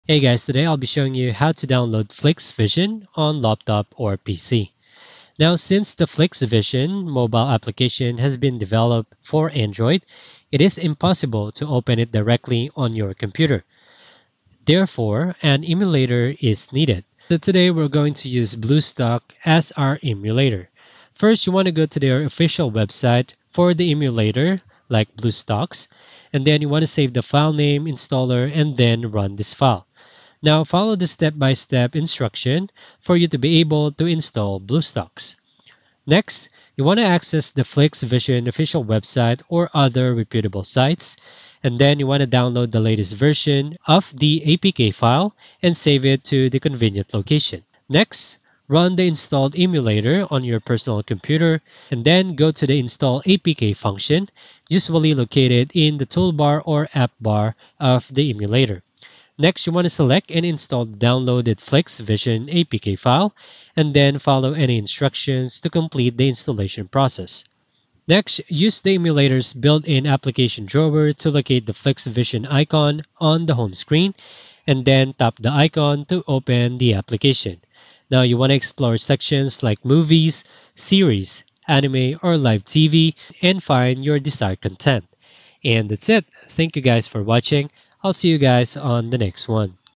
How To Download Flix Vision On Laptop Or PC 2025!
🏷️ Check Current Price on Amazon
🔖 Bookmark & Use for ANY Amazon Purchase (Supports Channel)
Flix Vision is a streaming platform that lets you watch movies and shows on any device. This guide shows how to download and install Flix Vision on your laptop or PC in 2025, including setup tips, system requirements, and login steps. Learn how to access your favorite content easily from your desktop.